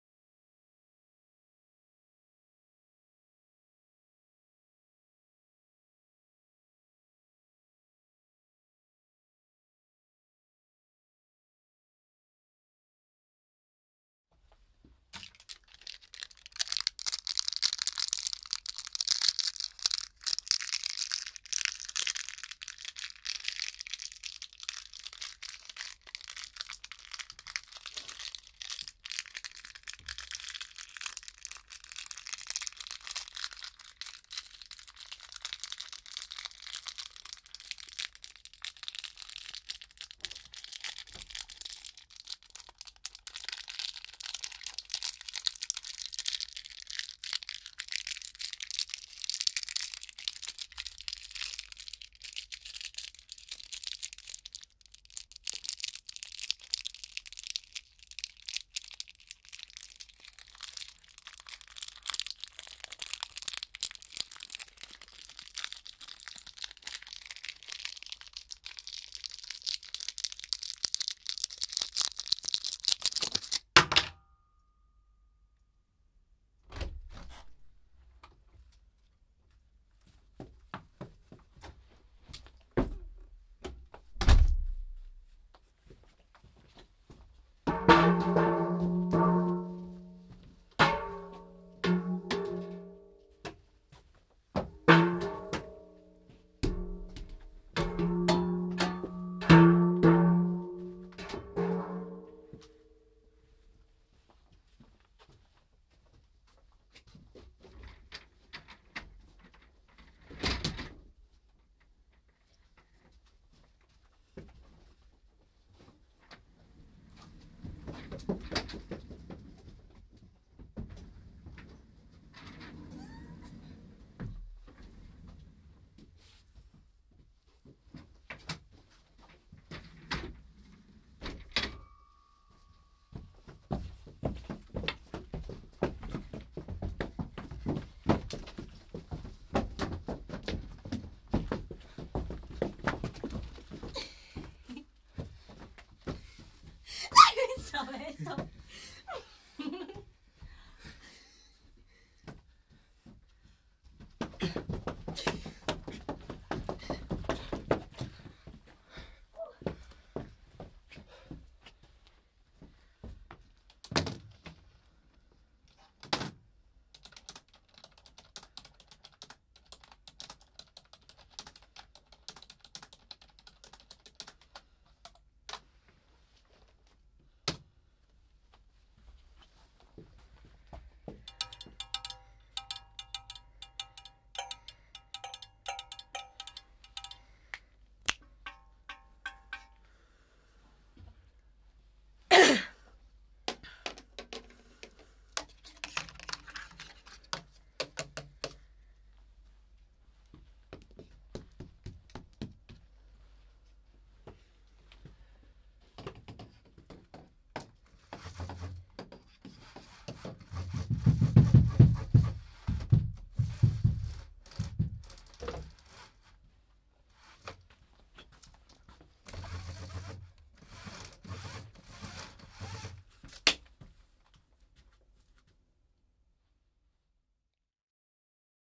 Binaural Recording: Dead Room
Experimenting with a pair of binaural microphones and a dummy head, Bebe and I recorded a host of arbitrary noises in an acoustically dead environment. Please note, headphones must be worn for the effect to be fully realised.